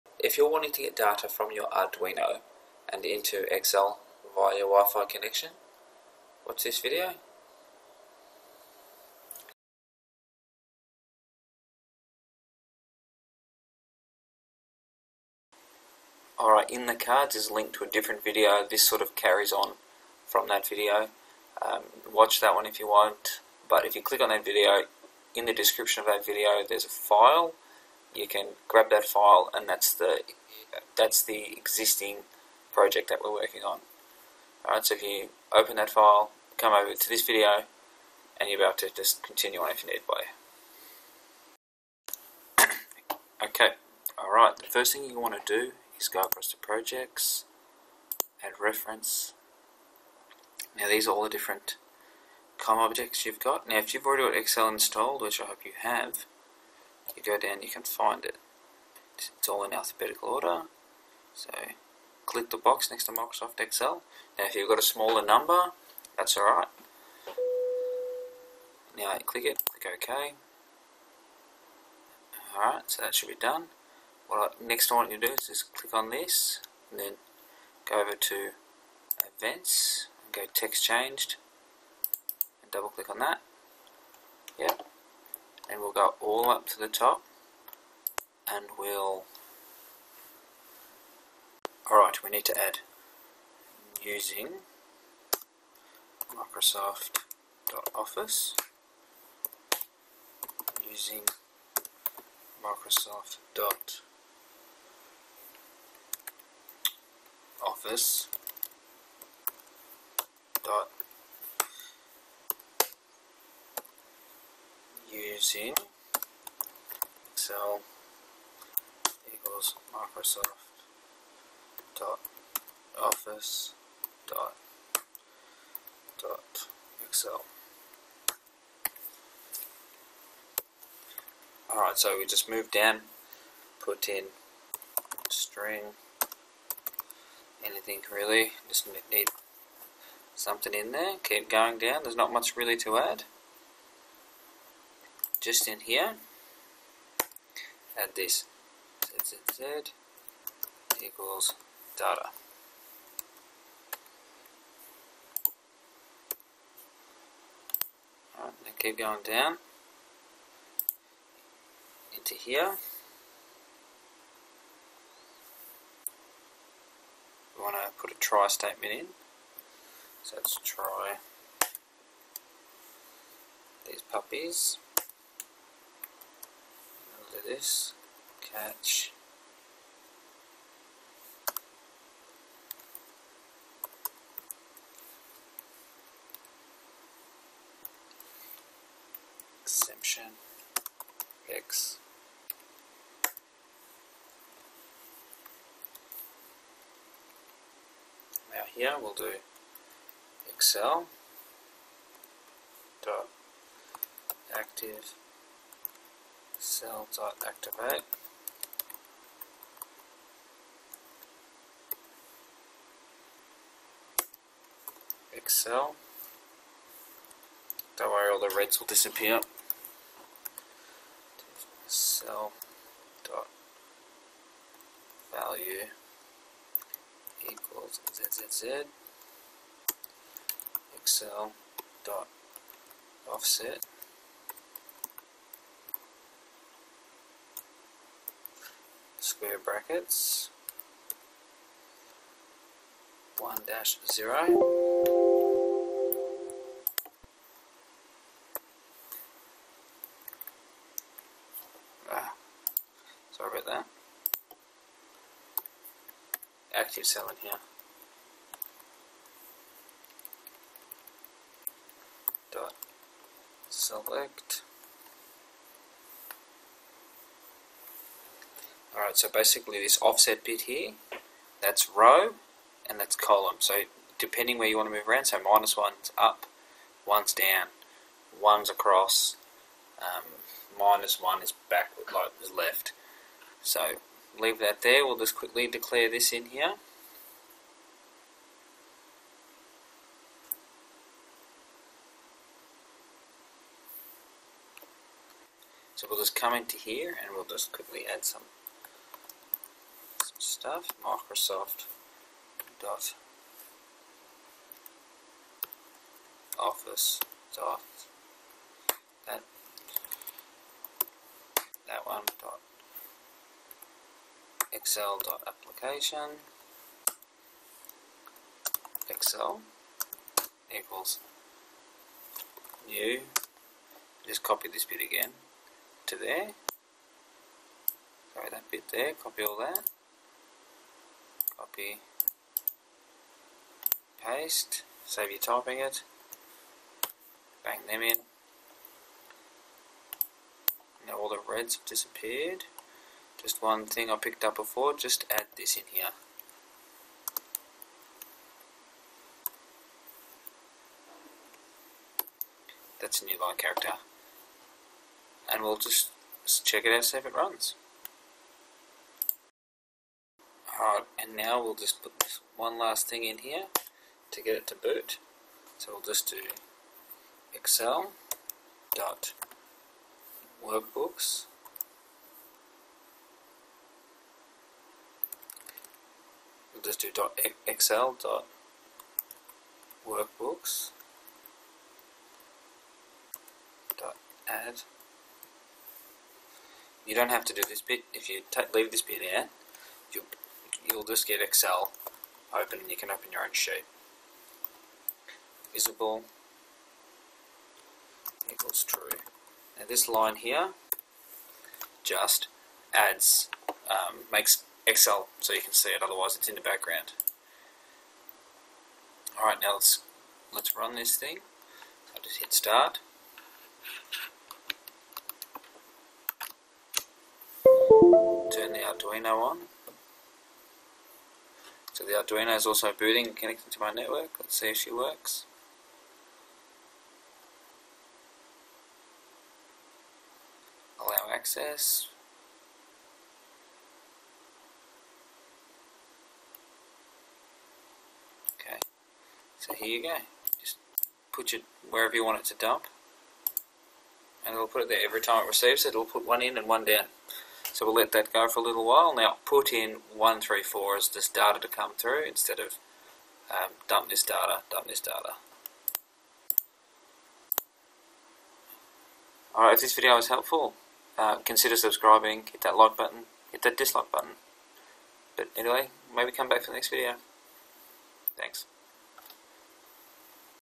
Arduino Wifi Data Collection in Excel Using C#
Collect data from your wifi connected Arduino into excel
using C#